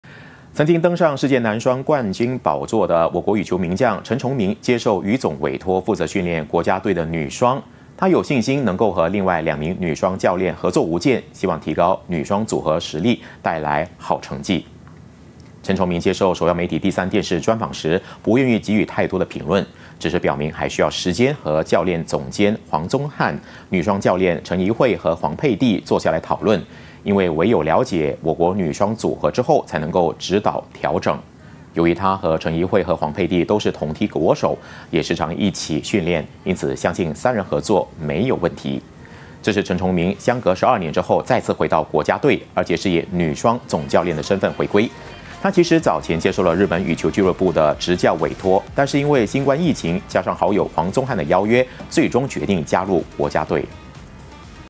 暌违12年重返国羽 陈重名任女双主教练
曾经登上世界男双冠军宝座的我国羽球名将陈重名接受羽总委托，负责训练国家队的女双，他有信心能够和另外两名女双教练合作无间，希望提高女双组合实力，带来好成绩。